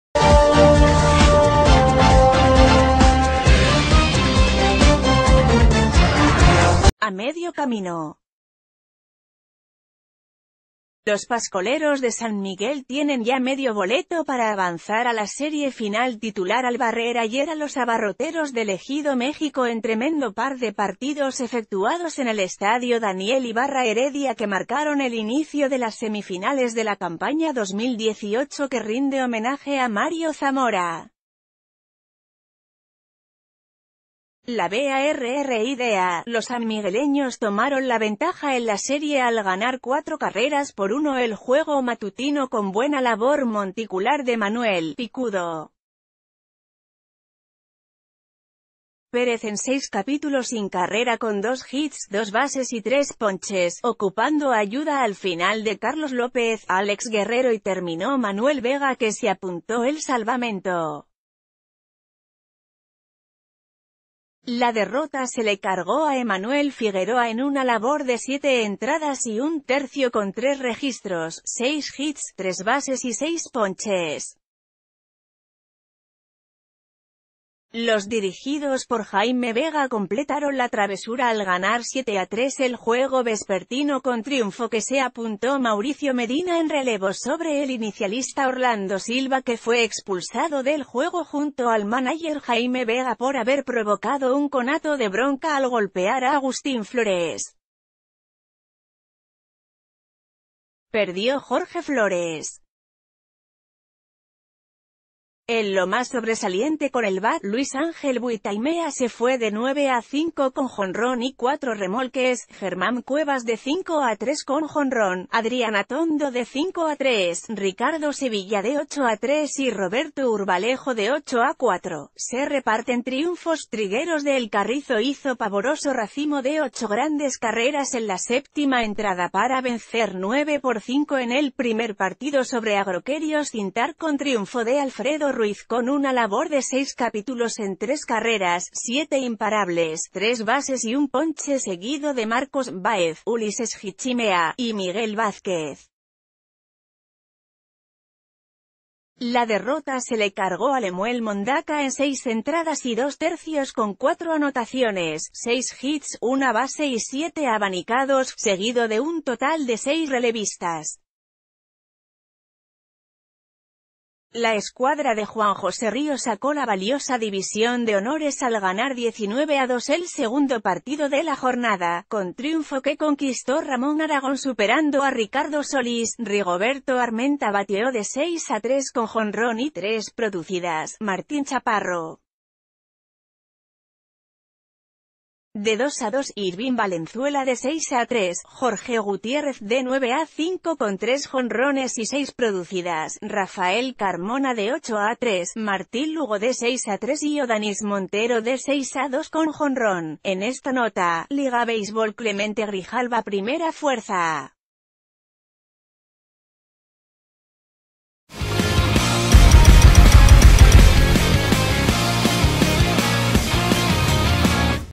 San Miguel con medio boleto
A medio camino. Los Pascoleros de San Miguel tienen ya medio boleto para avanzar a la serie final titular al barrer ayer a los Abarroteros del ejido México en tremendo par de partidos efectuados en el estadio Daniel Ibarra Heredia que marcaron el inicio de las semifinales de la campaña 2018 que rind...